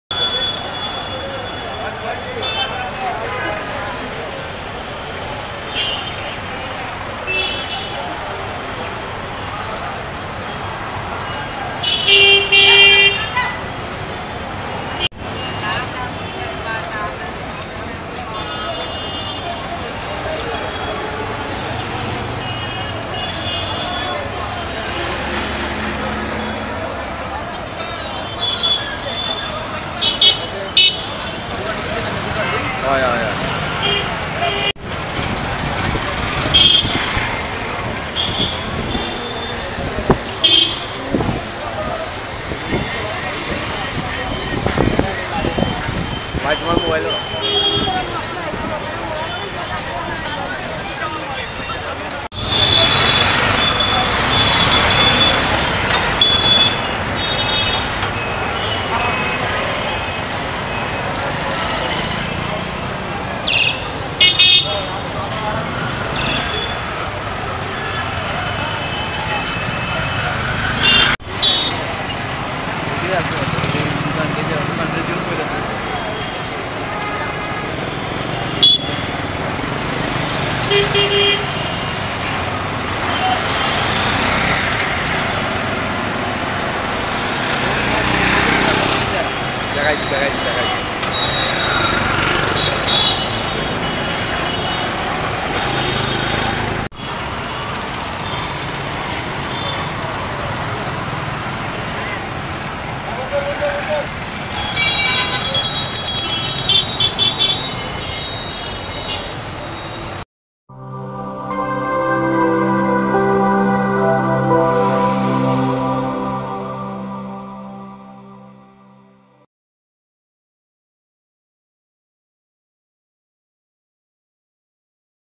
সোমবার থেকে ঘোষিত সীমিত পরিসরে লকডাউনে সিলেট নগরীর চিত্র। 28.06.2021
সোমবার থেকে ঘোষিত সীমিত পরিসরে লকডাউনে সিলেট নগরীর চিত্র।